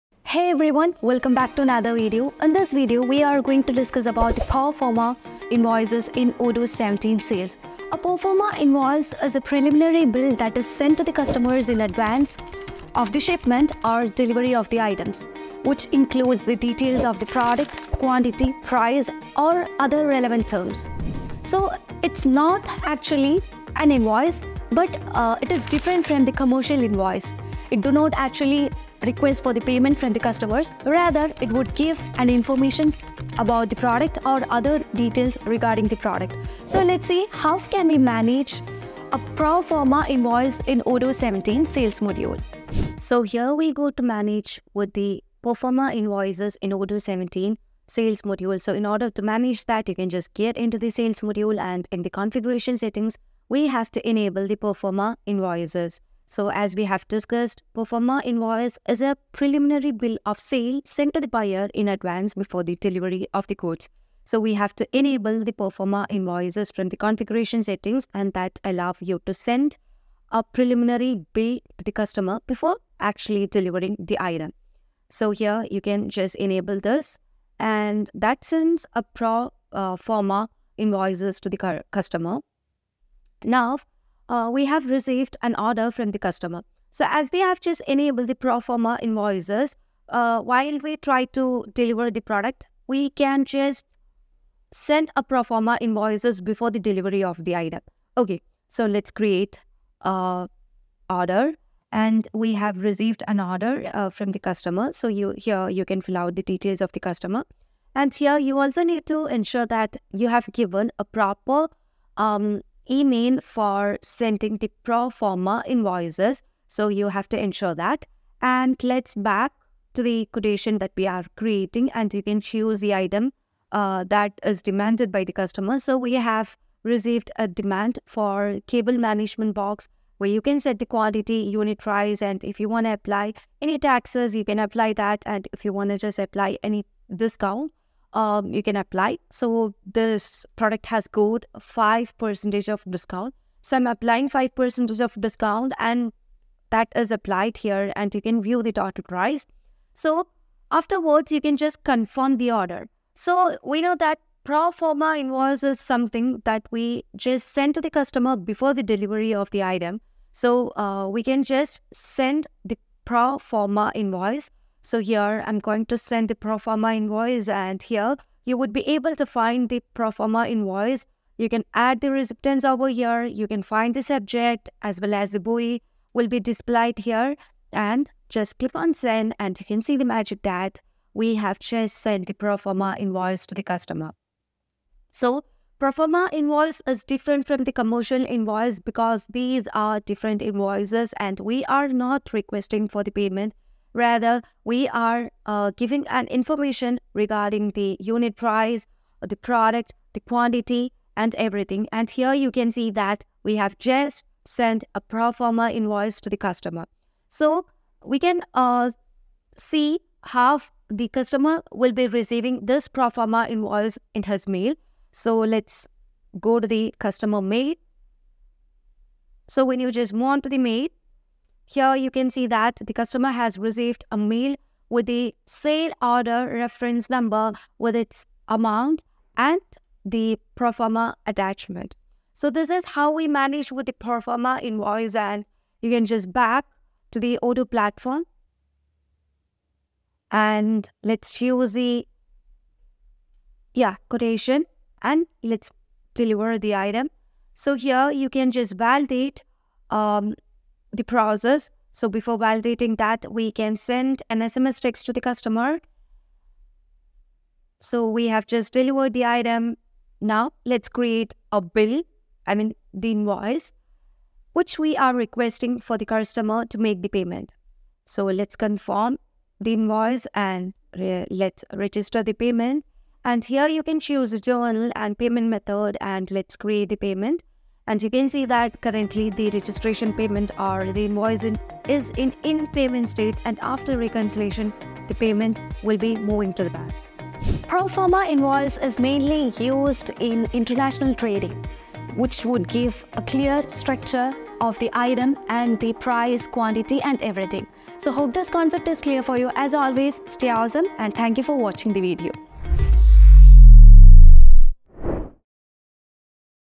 #39 How to Send Pro Forma Invoice to Your Customers in Odoo 17 | Proforma Invoice in Odoo 17 Sales
A proforma invoice is a preliminary bill of sale sent to buyers in advance of a shipment or delivery of goods. It provides detailed information about the products or services to be provided, including the quantity, price, and other relevant terms.

However, unlike a commercial invoice, a proforma invoice is not a demand for payment but rather a way to give the buyer an idea of the cost of the goods or services. Proforma invoices are often used in international trade, allowing the buyer to understand the cost implications before the actual goods are shipped.

It serves as a tool for customs, helping to determine duties and taxes. Keep in mind that a proforma invoice is not a legally binding document, and it does not replace a formal commercial invoice, which is used for actual billing purposes.